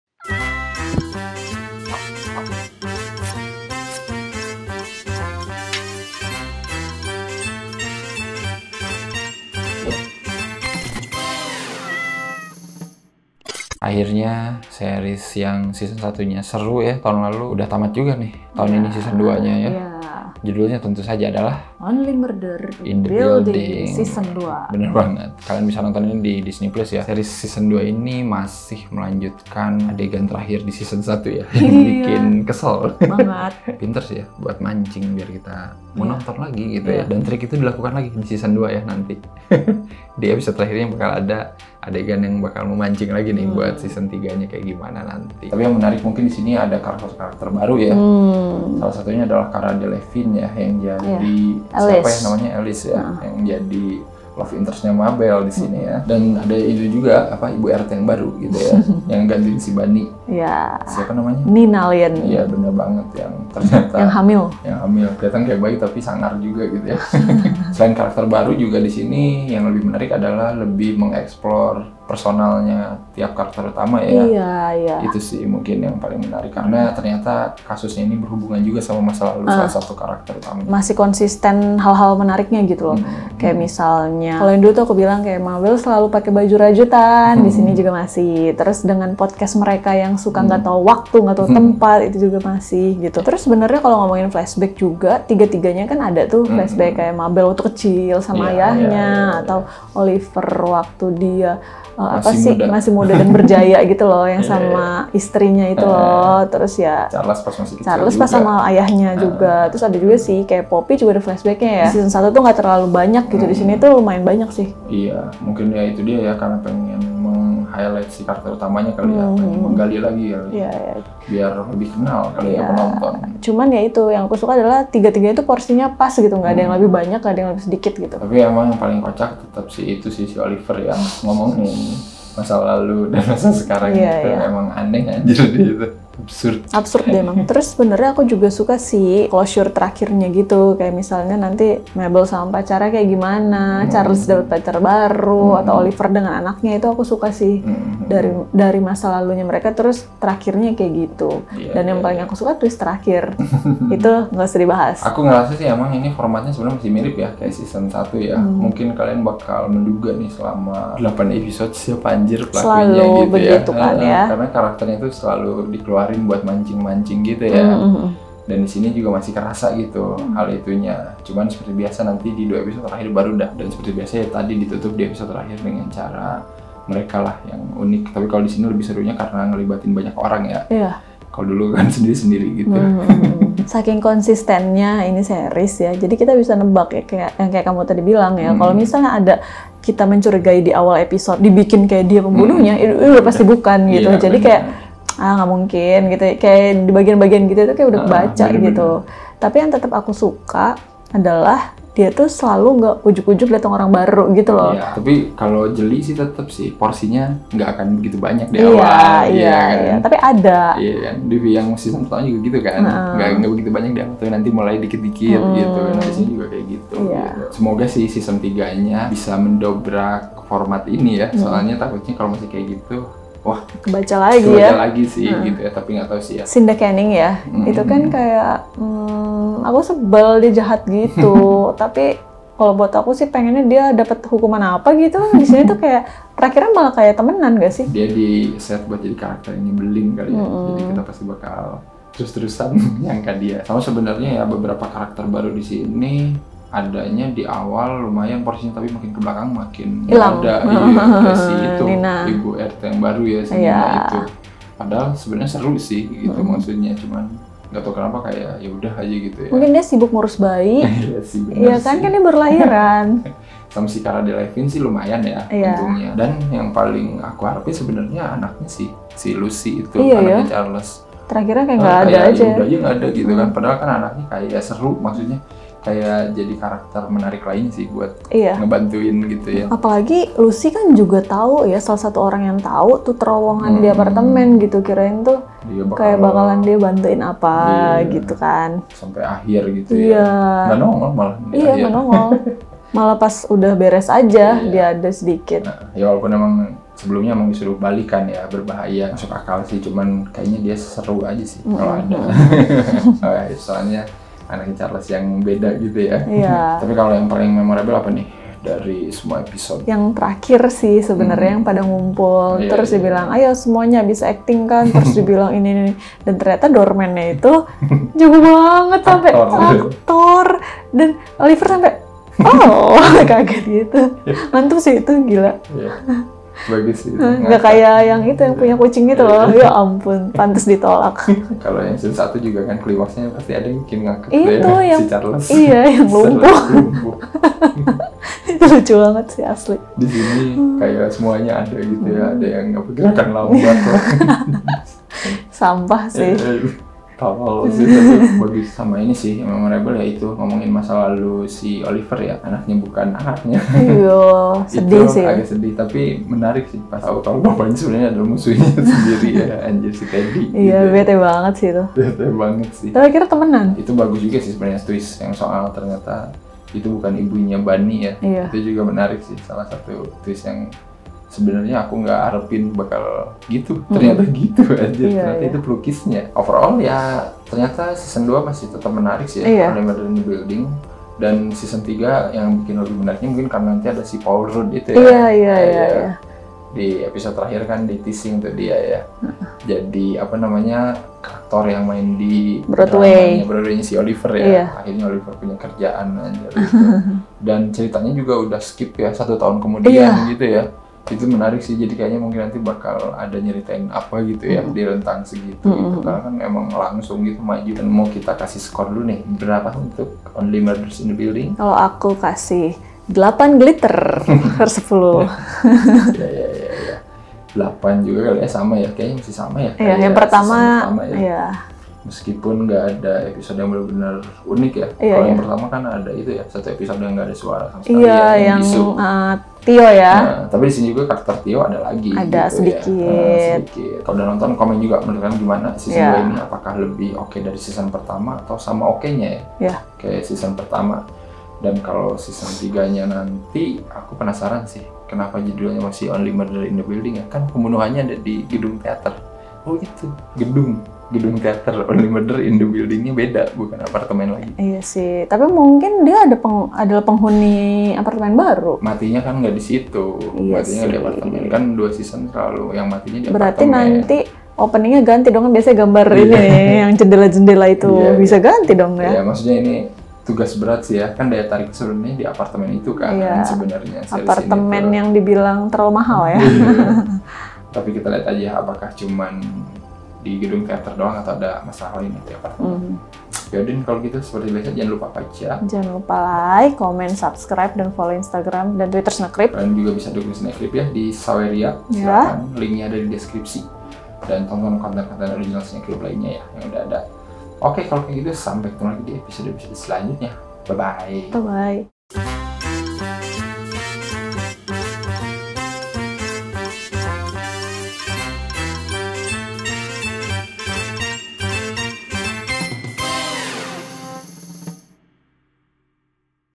Review Only Murders in the Building Season 2, Masih Menarik dan Masih Bikin Ngakak
Jadi investor kami yuk! Seikhlasnya aja, biar kami semangat terus bikin konten baru yang berkualitas buat kamu :) klik aja di sini

OnlyMurdersInTheBuilding membuktikan kalo serial misteri dengan konsep whodunit tidak harus selamanya serius dan mencekam. Setelah musim pertamanya yang sukses tahun lalu, kini Charles (SteveMartin), Oliver (MartinShort), dan Mabel (SelenaGomez) kembali lagi dengan musim keduanya yang masih tetap menarik dan bikin ngakak.

Kali ini mereka harus mencari tau siapa pembunuh Bu RT di apartemen Arconia yang mewah dan overpriced tersebut. Sambil mencari pembunuhnya, mereka bertiga juga harus berhadapan dengan masa lalu yang datang menghantui dan juga beberapa karakter baru yang mencurigakan.

Serial berjumlah 10 episode ini masih memakai pendekatan yang sama dengan musim pertamanya. Bagi penonton lama mungkin akan mudah menerka siapa dalang pembunuhan di kasus yang baru ini. Tapi setidaknya trio detektif ala-ala sekaligus siniar independen ini masih bisa memberikan beberapa kejutan yang dijamin tidak akan bikin bosan penonton sampai akhir episode.

Langsung aja tonton review-nya di cinecrib.

0:00 - Intro
1:07 - Masih konsisten dan lebih ngulik masa lalu karakter utama
2:17 - Tiap karakter diberi closure yang pas
2:35 - Treatment series masih sama kayak season pertama
3:03 - Tapi jadinya agak ketebak
3:46 - Karakter Tina Fey masih ngeselin
4:03 - Screen time beberapa karakter baru kurang banyak
5:06 - SPOILER
6:47 - CINESCORE!

Host: @ariasparrow @anthywf